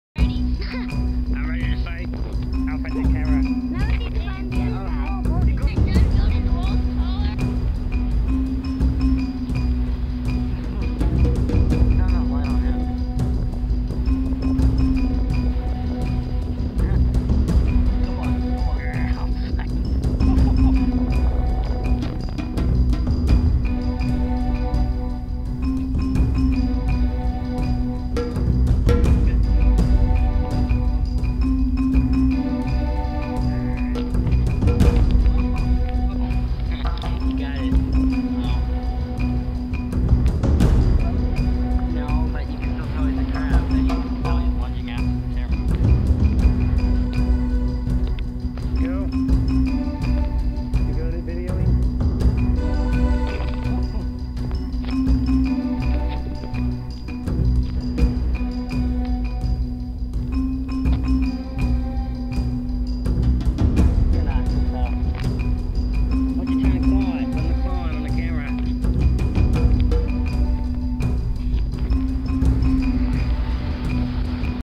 Ghost Crab Attacking My Camera
This brave little guy was ready to FIGHT!! I did not harm the crab in any way, nor was my camera or fingers harmed.